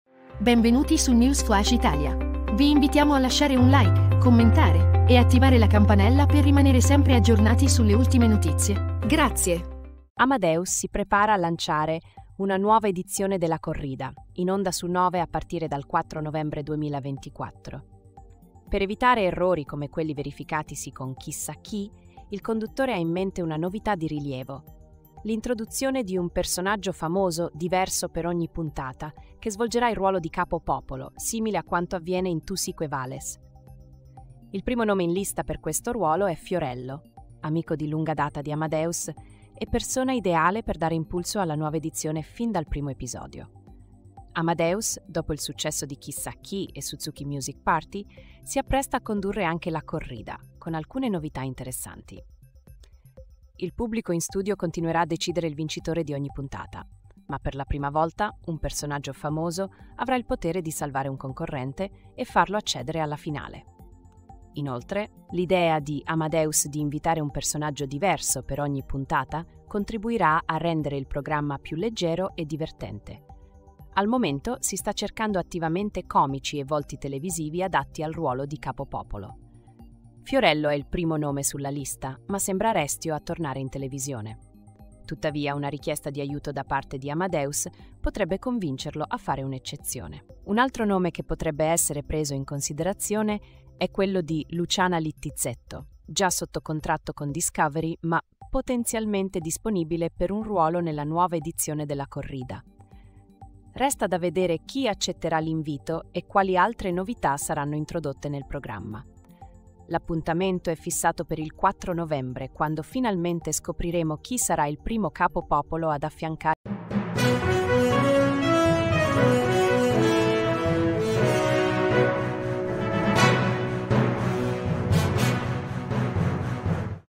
FIORILLO SALVERÀ I CONCORRENTI? AMADEUS RIVOLUZIONA LA CORRIDA SU NOVE
Amadeus si prepara a lanciare una nuova edizione de La Corrida, in onda su Nove a partire dal 4 novembre 2024. Per evitare errori come quelli verificatisi con "Chissà chi", il conduttore ha in mente una novità di rilievo: l'introduzione di un personaggio famoso diverso per ogni puntata, che svolgerà il ruolo di capo popolo, simile a quanto avviene in "Tú sí que vales". Il primo nome in lista per questo ruolo è Fiorello, amico di lunga data di Amadeus e persona ideale per dare impulso alla nuova edizione fin dal primo episodio.

Amadeus, dopo il successo di "Chissà chi" e "Suzuki Music Party", si appresta a condurre anche La Corrida, con alcune novità interessanti. Il pubblico in studio continuerà a decidere il vincitore di ogni puntata, ma per la prima volta un personaggio famoso avrà il potere di salvare un concorrente e farlo accedere alla finale. Inoltre, l'idea di Amadeus di invitare un personaggio diverso per ogni puntata contribuirà a rendere il programma più leggero e divertente.

Al momento, si sta cercando attivamente comici e volti televisivi adatti al ruolo di capo popolo. Fiorello è il primo nome sulla lista, ma sembra restio a tornare in televisione. Tuttavia, una richiesta di aiuto da parte di Amadeus potrebbe convincerlo a fare un'eccezione. Un altro nome che potrebbe essere preso in considerazione è quello di Luciana Littizzetto, già sotto contratto con Discovery ma potenzialmente disponibile per un ruolo nella nuova edizione de La Corrida.

Resta da vedere chi accetterà l'invito e quali altre novità saranno introdotte nel programma. L'appuntamento è fissato per il 4 novembre, quando finalmente scopriremo chi sarà il primo capo popolo ad affiancare Amadeus in questa nuova avventura televisiva.